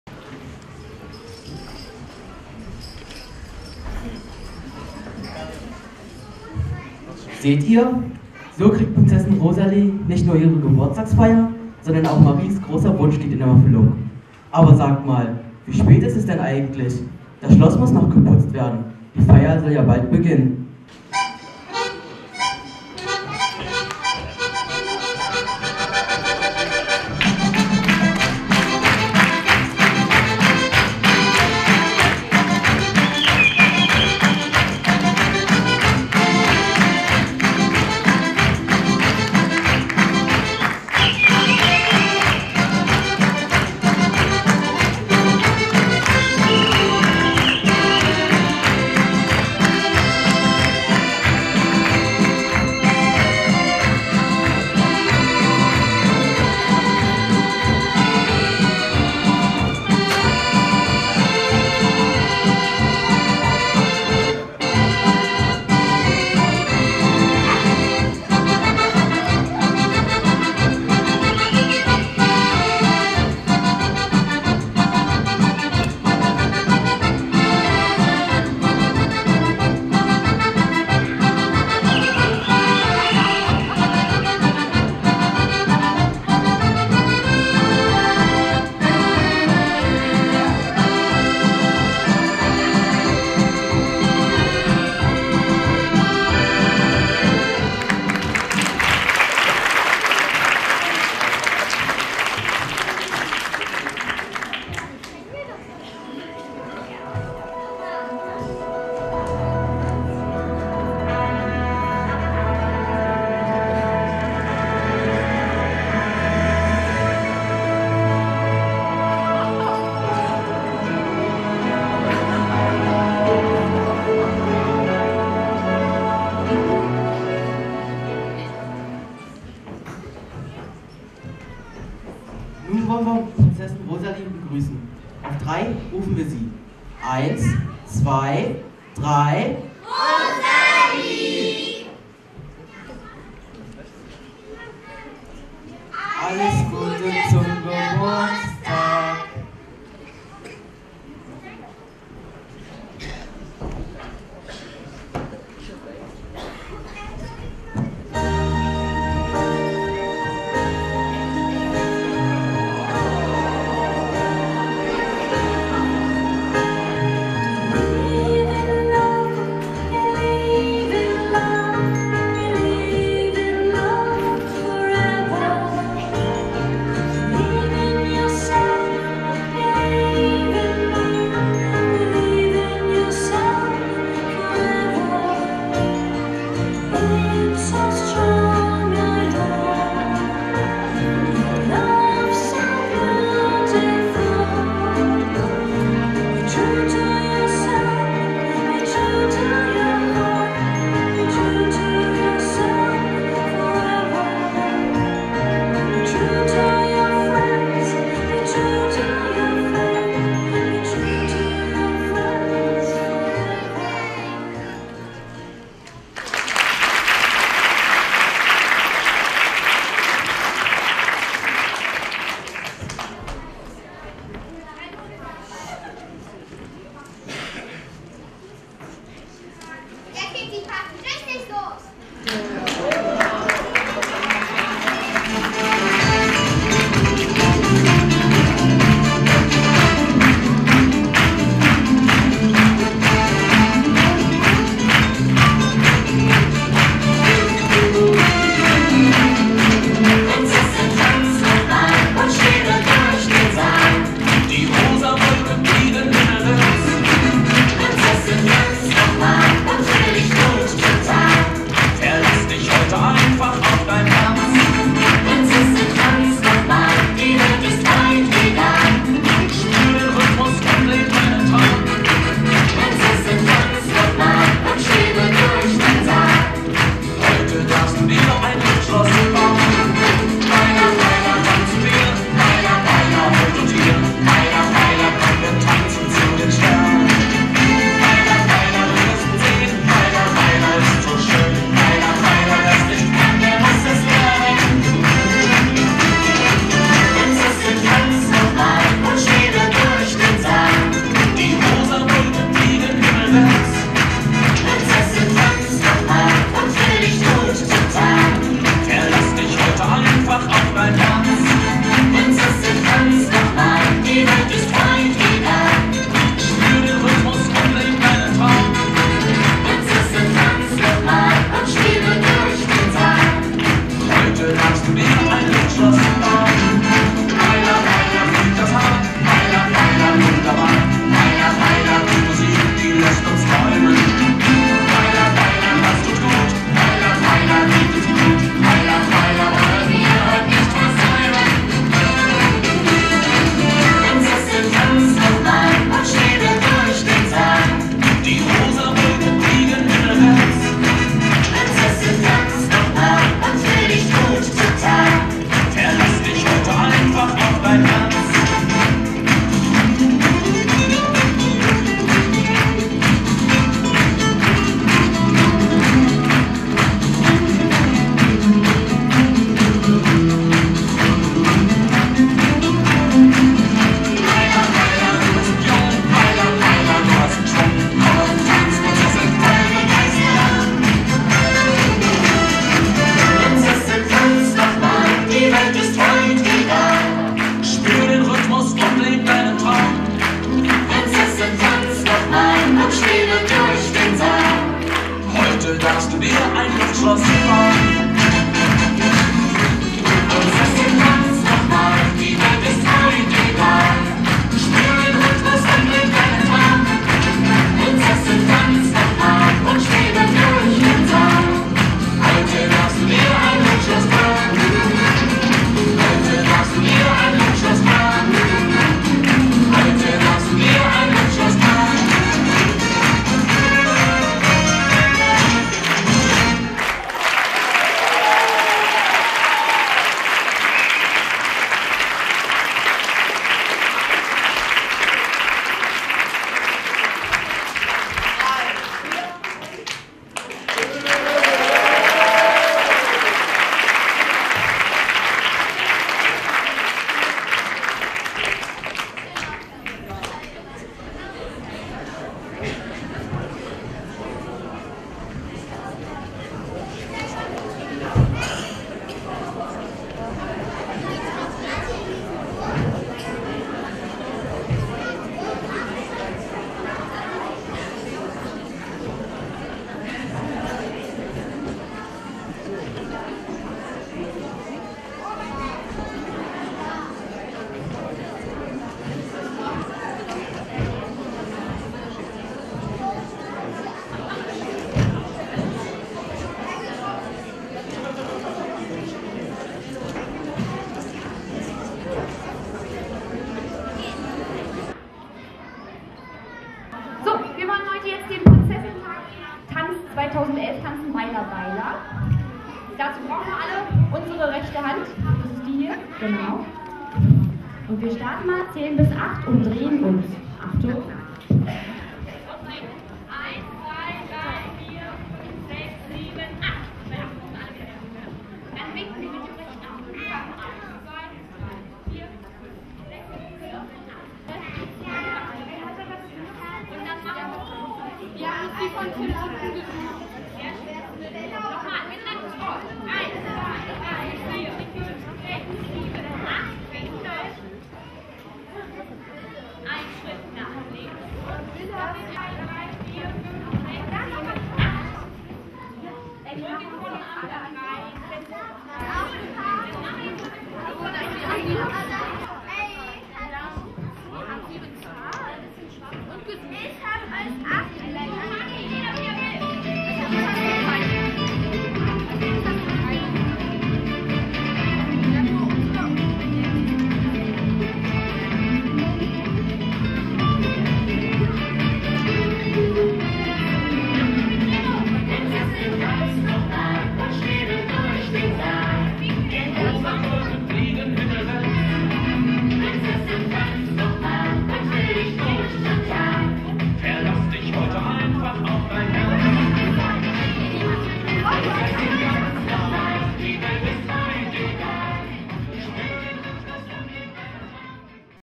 Disney Prinzessin-Tag 2011 der Tanzschule Röppnack - Teil 3
Am 24. September fand im OLI Kino Magdeburg der Bundesweite Prinzessin-Tag statt.

Die Schüler der Tanzschule Röppnack zeigten Ihr Können.